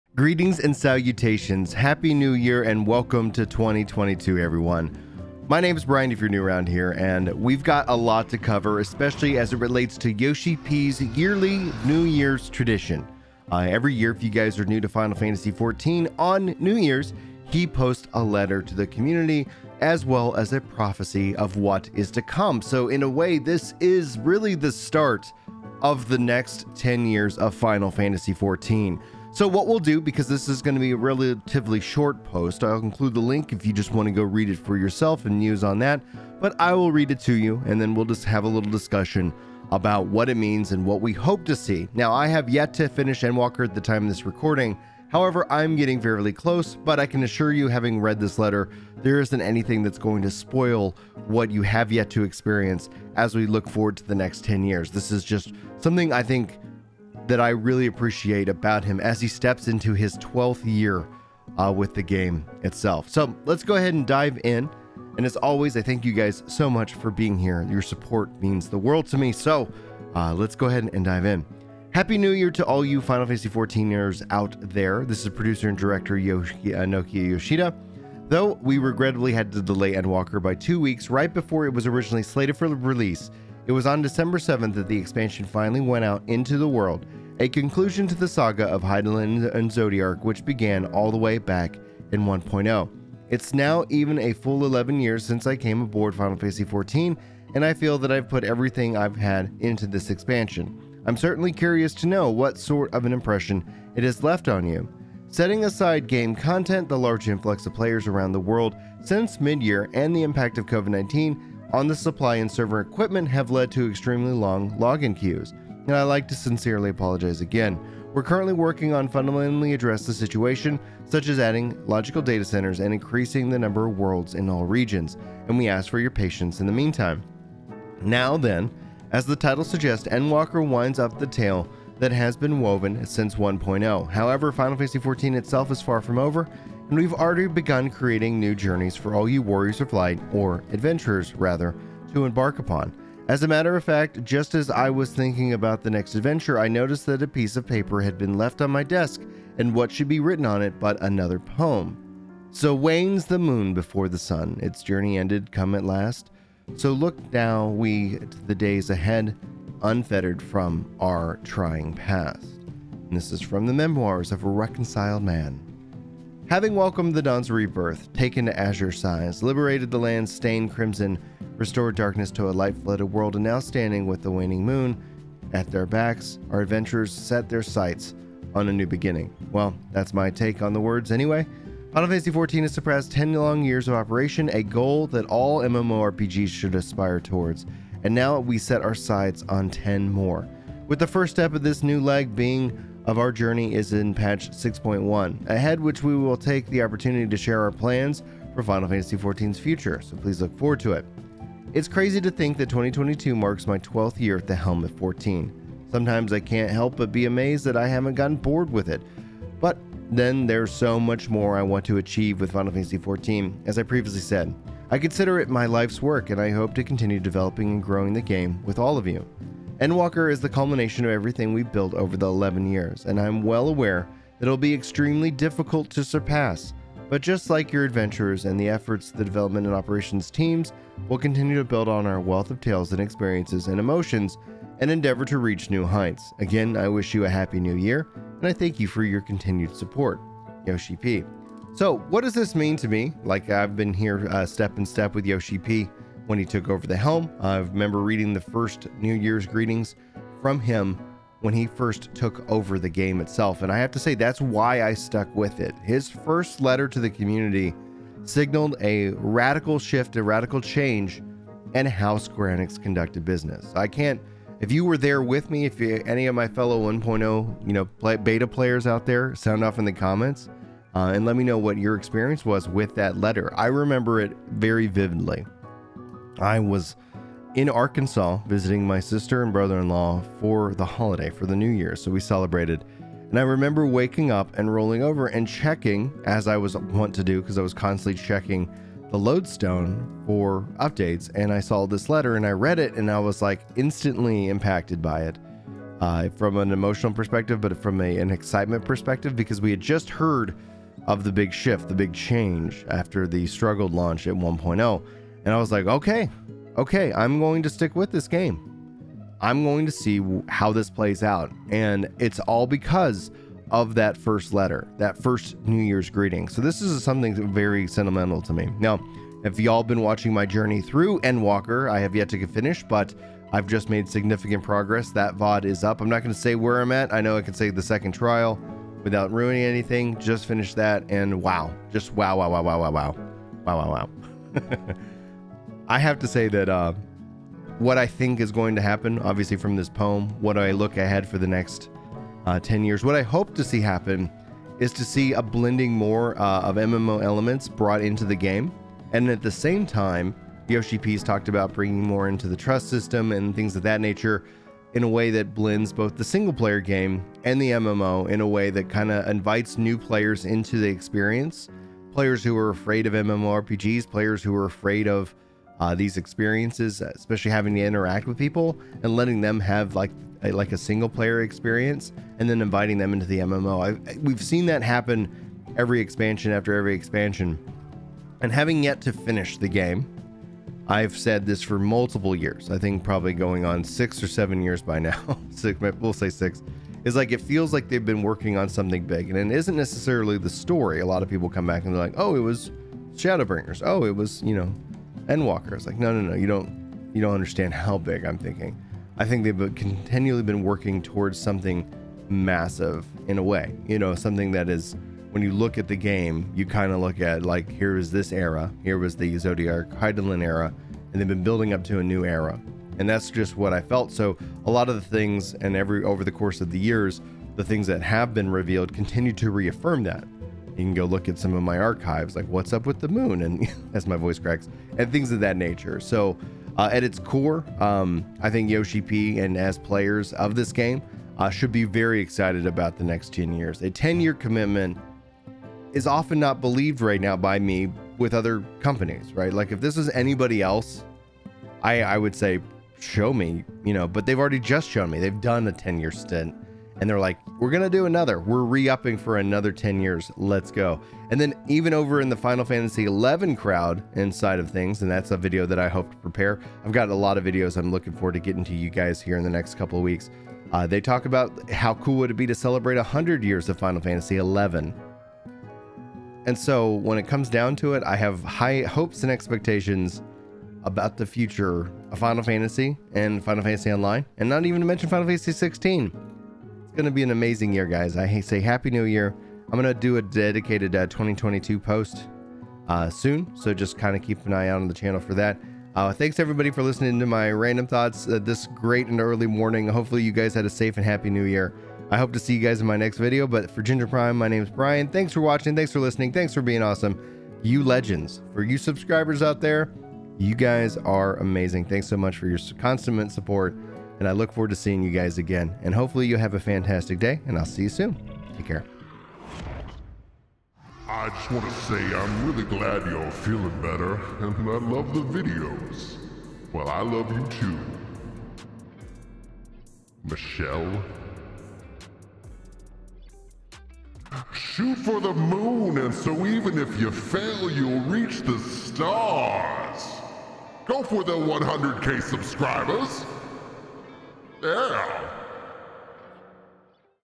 Yoshi-P's New Year's Greetings & FFXIV Endwalker's Next 10 Years
Final Fantasy XIV's Yoshi-P shares his new Year's Greetings and hints at the next 10 years of FF14

Ginger Prime is an MMORPG Channel

Outline
0:00 Introduction
1:00 New Years Greetings
4:30 The Next Ten Years of FFXIV
8:00 Final Thoughts &...